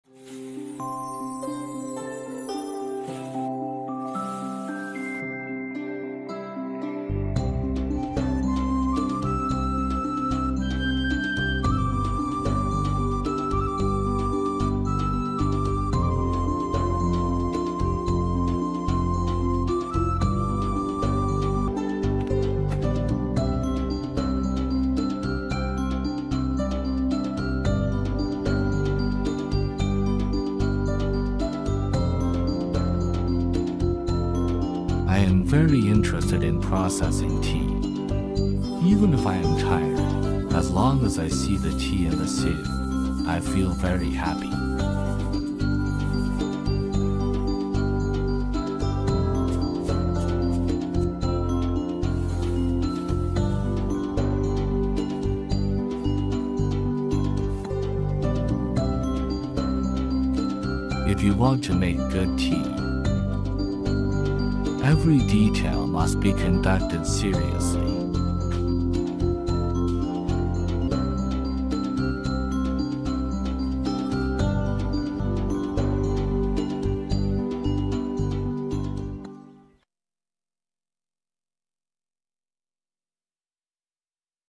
Mastering Keemun Black Tea - The Secrets of Xie Yongzhong [AI Voiceover]
Explore the rich tradition of Keemun Black Tea with our documentary, uniquely narrated using advanced AI dubbing technology by OneSound Media. Join us in Qimen County, where Tea Master Xie Yongzhong reveals the secrets behind one of the world's most esteemed teas.

✨ Experience the Future of Storytelling:

Discover the ancient craft of Keemun Black Tea through an AI-voiced narration, showcasing OneSound Media's cutting-edge technology.
Gain insights into Master Xie Yongzhong's expertise in creating this celebrated tea.

🔍 Why Watch?

Learn about traditional Keemun tea-making methods.
Understand the unique qualities making Keemun a global tea treasure.
Experience the synergy of traditional tea culture and modern AI technology.
This video is perfect for tea enthusiasts and tech aficionados alike. Witness how OneSound Media's AI dubbing adds a novel dimension to the storytelling, enhancing your understanding of Keemun Black Tea.

Your feedback is valuable – drop your thoughts and questions below!